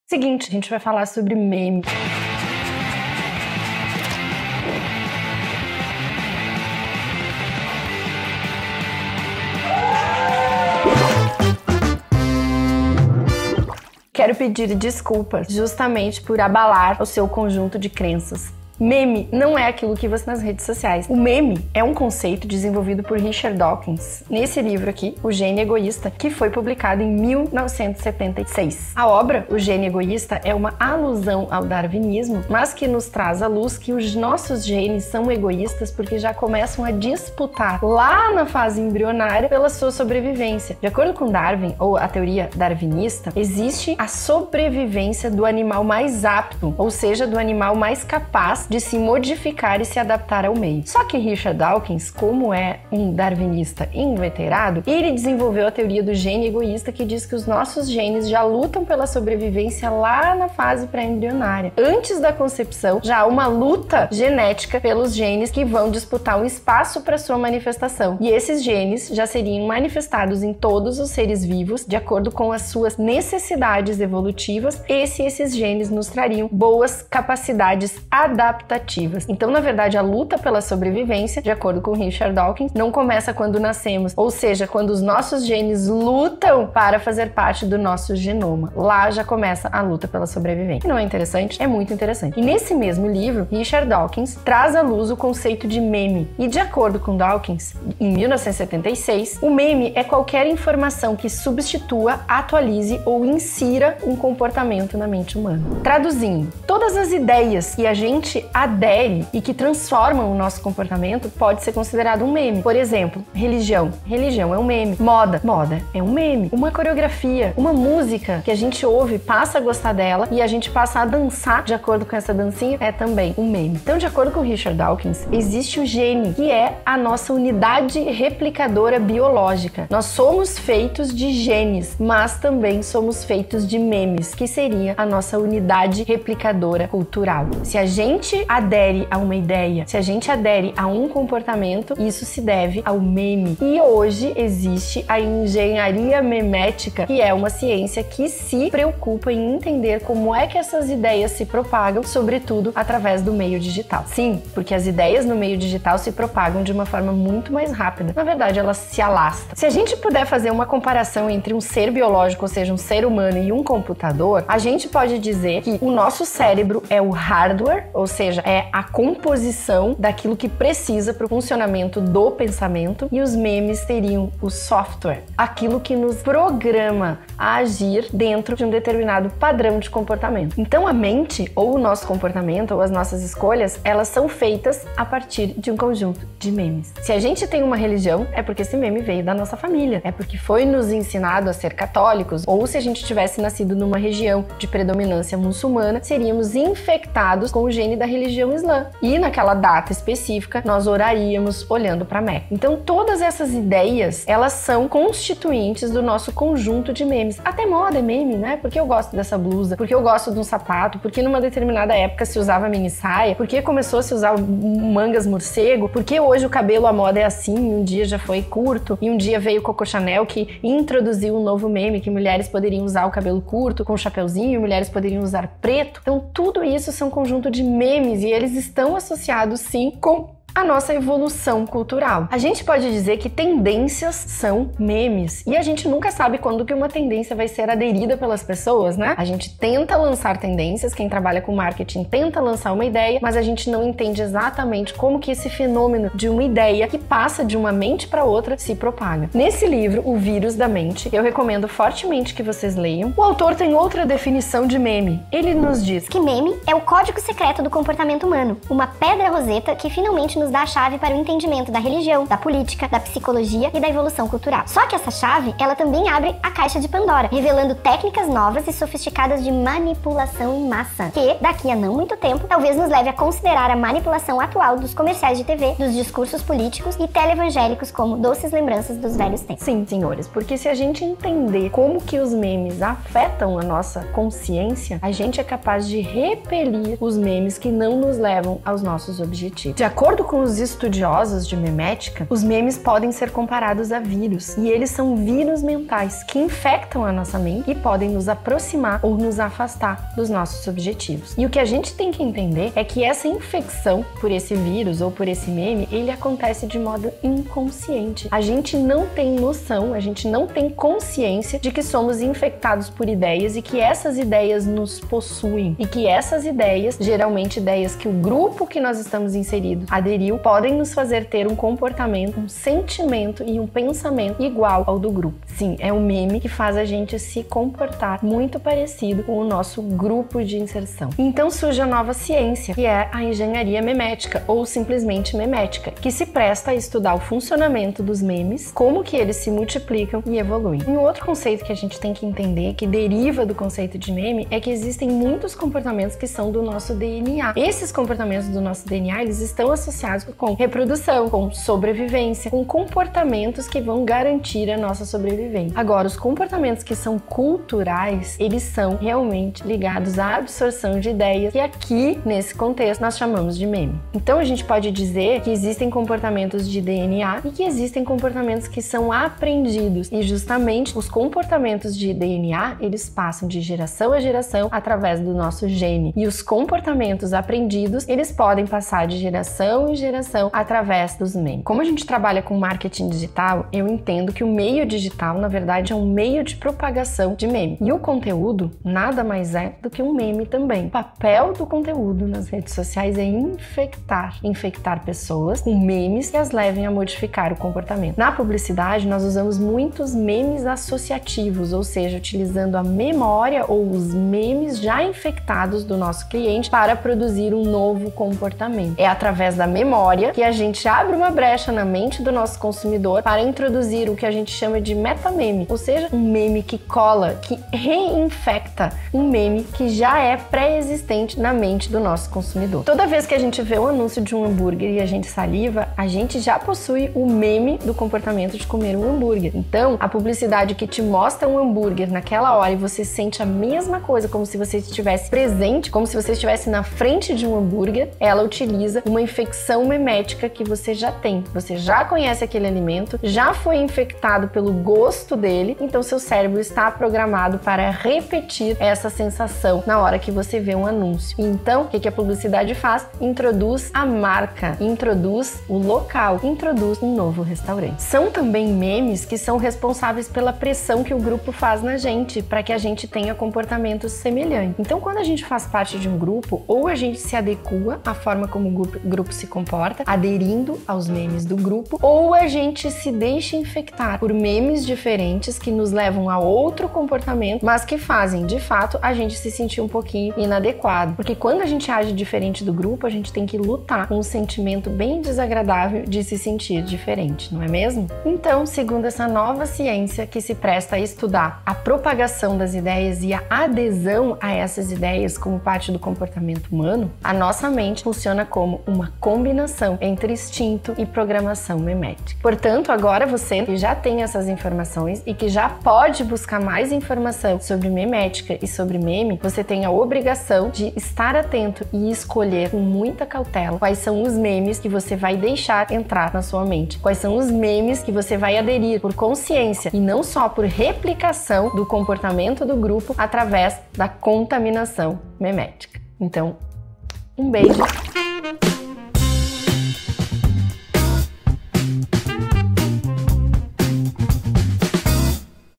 O QUE É MEME? Conheça a MEMÉTICA, a CIÊNCIA DOS MEMES. | Rejane Toigo
Memes estão por todas as partes, a internet tornou os memes parte do nosso dia a dia, mas você sabe o que é meme realmente?

Para entender os tipos de memes, qual a definição de meme e o que meme significa, você precisa entender que existe uma ciência dos memes, a memética. Além da origem da palavra meme, você vai entender como os memes influenciam nossa vida, vai conhecer o que é memética e com esse conhecimento, pode rever a forma de usar memes, pudendo incluir esse conceito inclusive na sua estratégia digital.

O QUE É MEME? Conheça a MEMÉTICA, a CIÊNCIA DOS MEMES.

TAGS